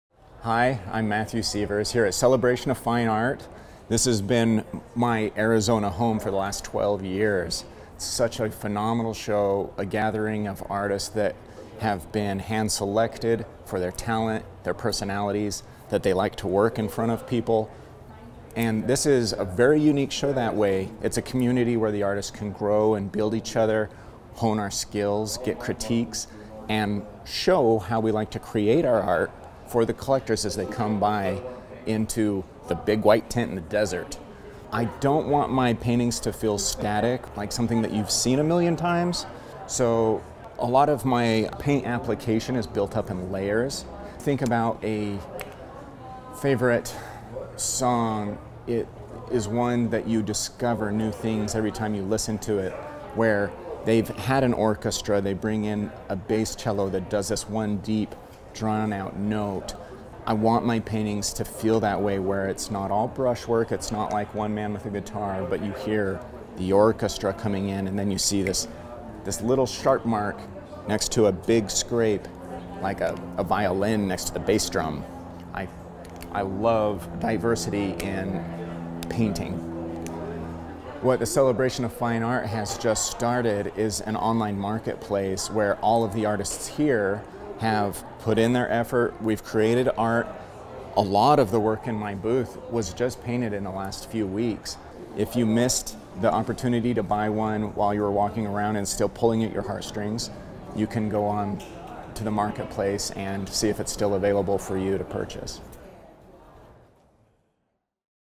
Discover Matthew Sievers and How His Work Is Not Unlike an Orchestra
Matthew Sievers builds layers in his paintings to not only achieve a distinct look, but create richness and depth. He likens it to an orchestra that pulls in various instruments to achieve a certain sound.